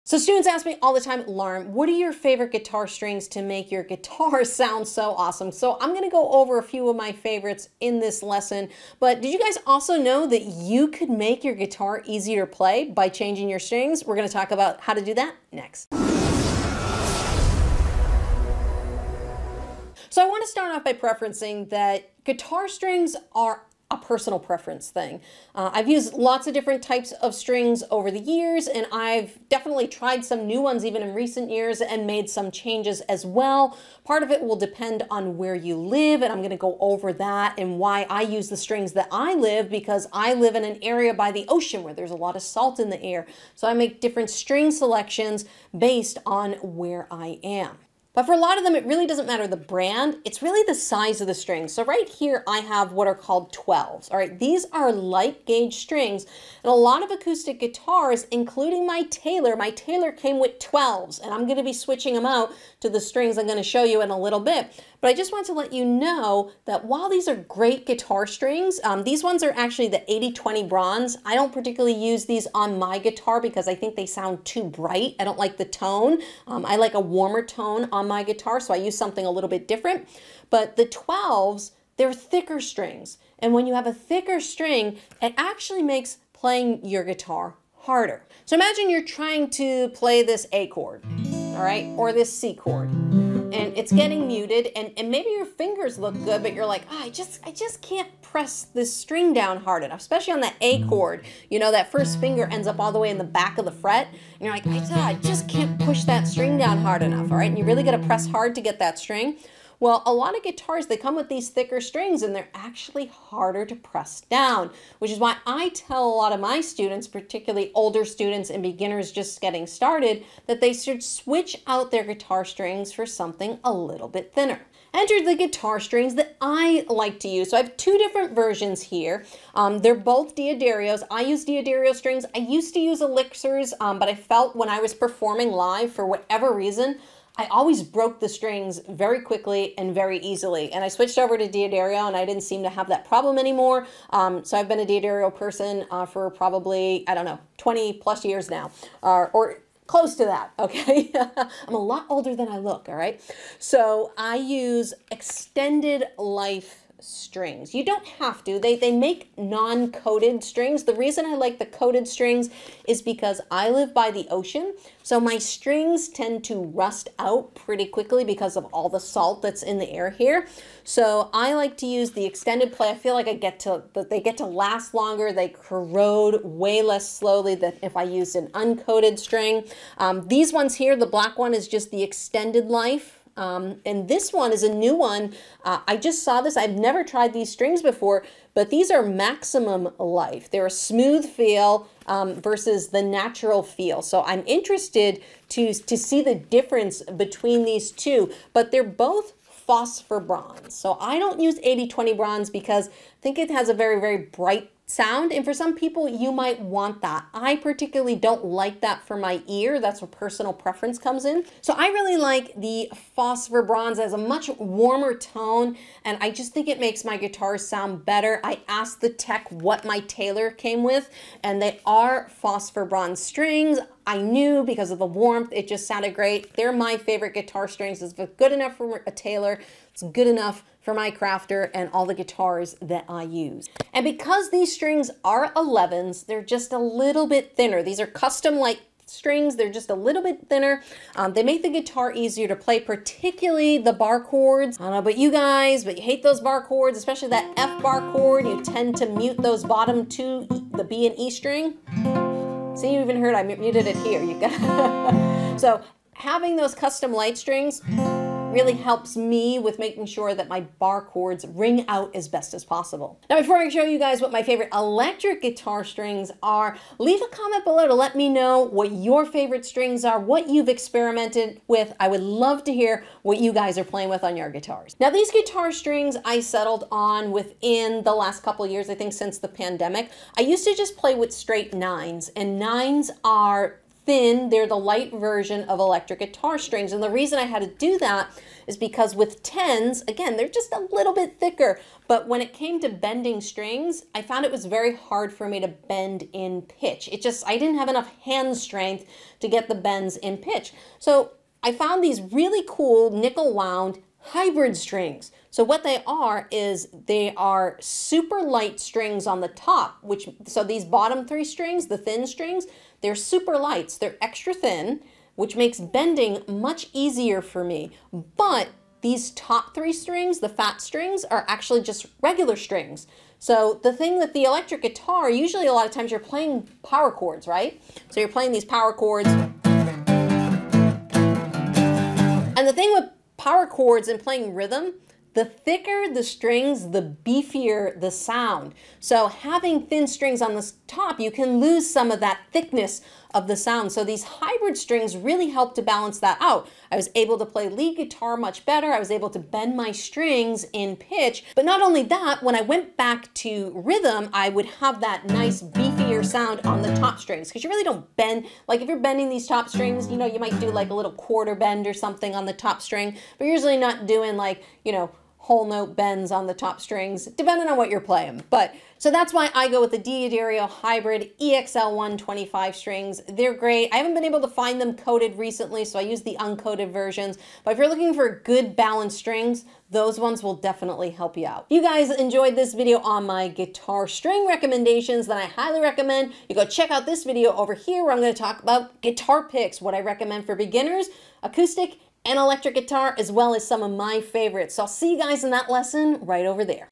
How I Get My WARM Guitar Tone & Make My Guitar EASIER To Play
Best Guitar Strings for Beginners - How I get my warm guitar sound while making my guitar easier to play. - Looking for the best guitar strings for beginners? Wish your guitar didn't sound so metallic? When I'm going to show you the exact guitar strings I use on my guitar to make playing chords (even barre chords!) easier to play. But not only that, I'm going to tell you about a very specific type of strings that will make your guitar sound beautiful and warm. 👉STRINGS RECOMMENDATIONS

⏱ Best Guitar Strings for Beginners Chapters: ⏱

0:00 - Intro
0:48 - Make Your Guitar Easier To Play
2:04 - The Guitar Strings That I Use
3:19 - The SECRET To My Warm Guitar Sound
4:34 - Favorite Electric Guitar Strings

Lauren :)